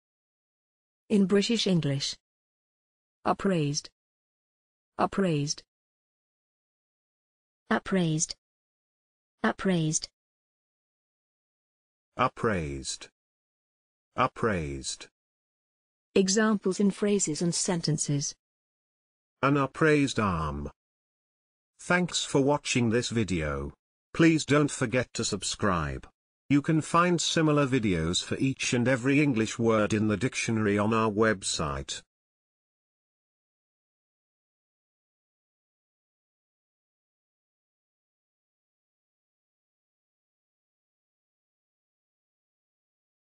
upraised - pronunciation + Examples in sentences and phrases
This is the pronunciation of upraised in British English + a few examples of its pronunciation in phrases and sentences.